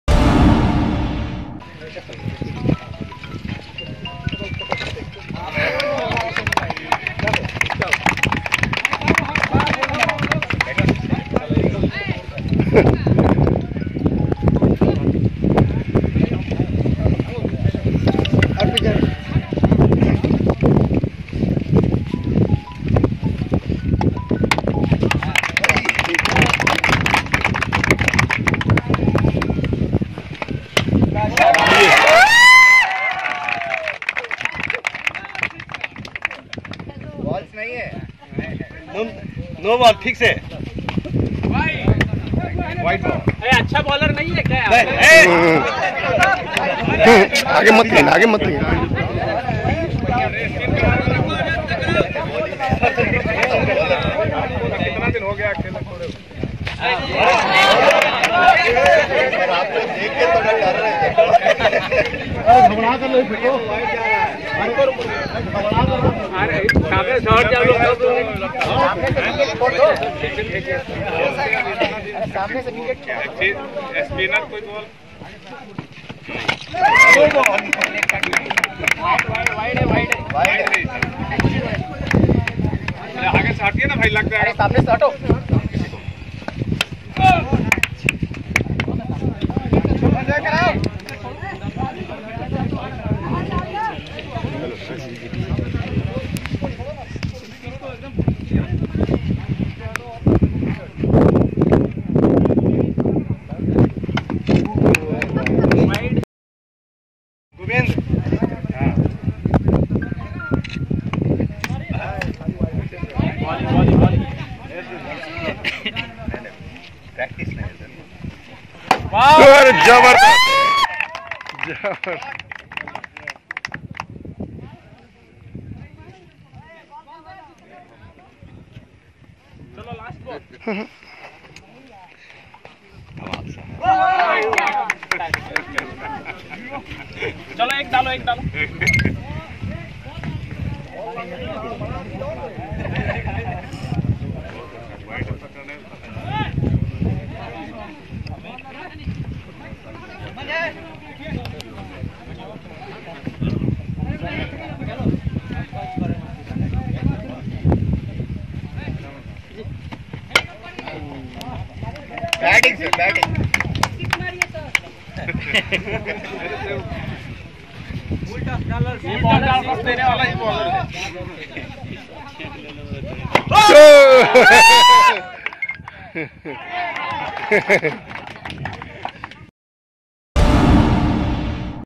मोतिहारी DM शीर्षत कपिल अशोक केसरिया के युवाओं संग क्रिकेट खेलते देखे गए
HTV News  Exclusive